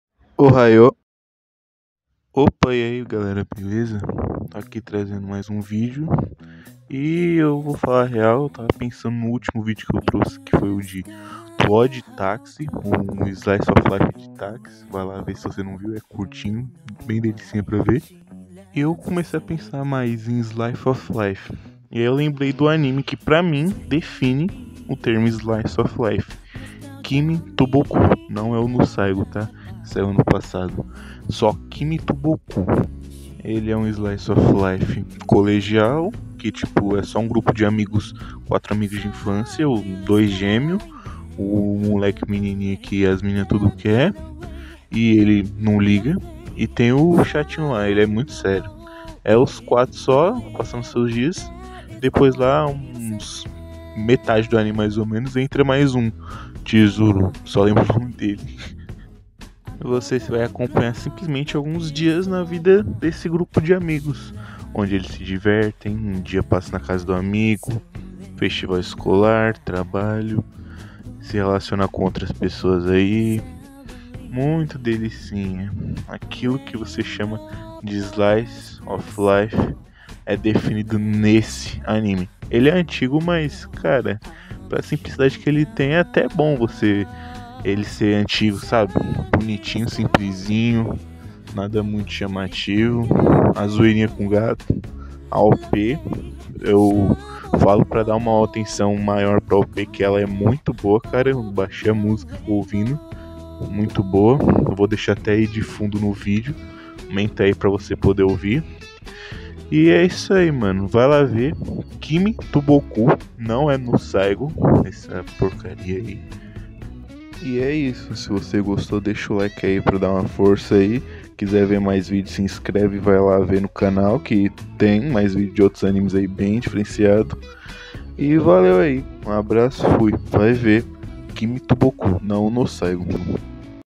O QUE É SLICE OF LIFE? MAICOTAKU RECOMENDA - KIMI TO BOKU (NO SAIGO NÃO)
o anime q melhor representa pra mim o gênero slice of life, kimi to boku, não é o kimi to boku no saigo!